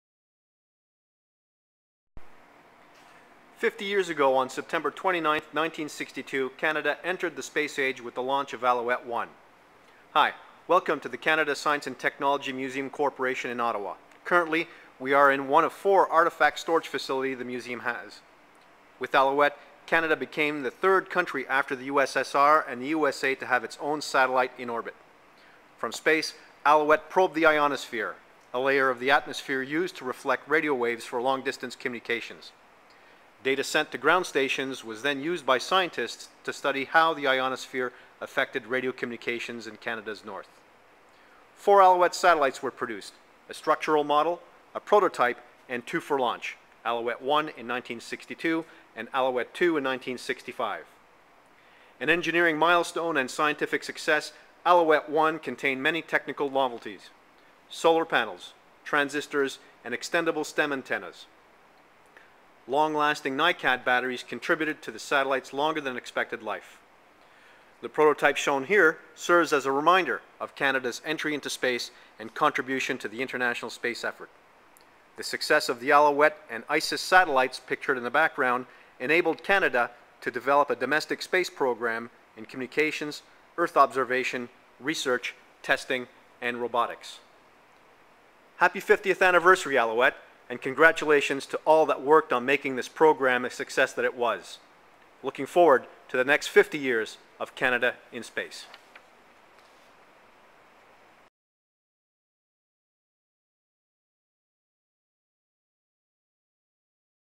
The 50th Anniversary of the Launch of Alouette-I
Alouette-I Prototype, 1961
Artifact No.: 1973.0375
Source: Defence Research Telecommunications Establishment, now Communications Research Centre Canada

Alouette-1 was launched September 29, 1962 by a Thor-Agena rocket from Vandenberg Air Force Base in California, USA, marking Canada's entry into space.

Canada became the third country after the USSR and the USA to have a satellite of its own in orbit.

From space, Alouette probed the ionosphere, a layer of the atmosphere used to reflect radio waves for long distance communications.

Data sent to ground stations was used by scientists to study how the ionosphere affected radio communications in Canada's North.

Canada's $1 win celebrates the little satellite that could

(aussi disponible en français)